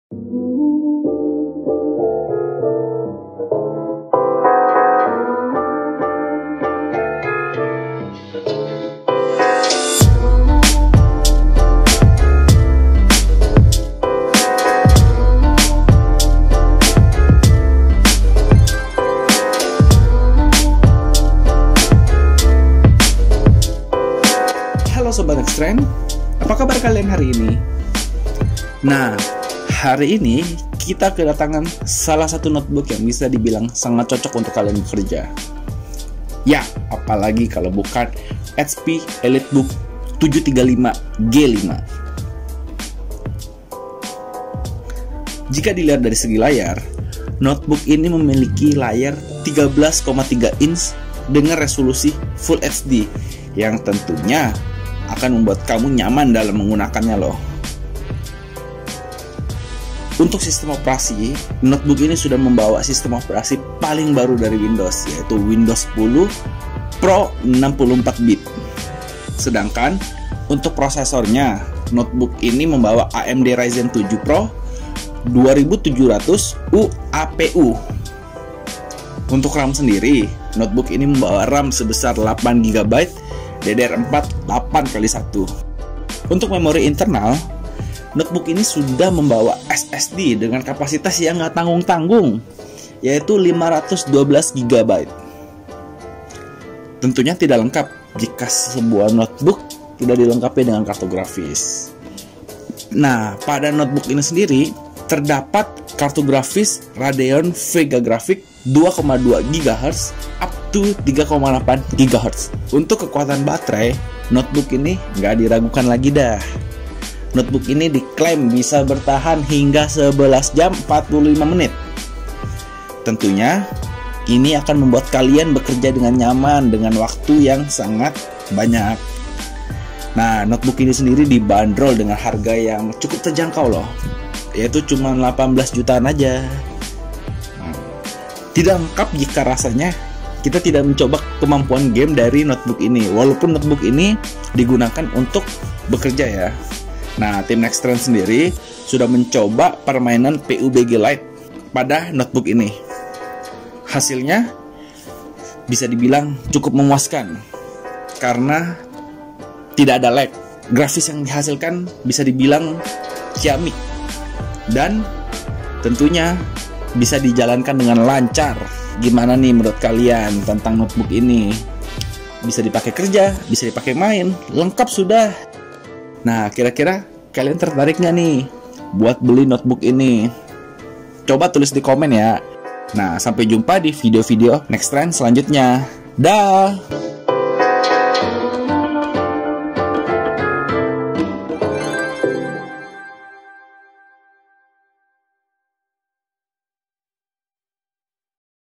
Review Notebook HP Elitebook 735 G5, Bisa Dipakai Main Juga?
Review Notebook HP Elitebook 735 G5 cukup mengejutkan loh.

Ternyata tak hanya cocok untuk bekerja saja, notebook ini juga bisa digunakan untuk bermain loh.

Selain itu, banyak keunggulan serta kelebihan lain dari Notebook HP Elitebook 735 G5 ini.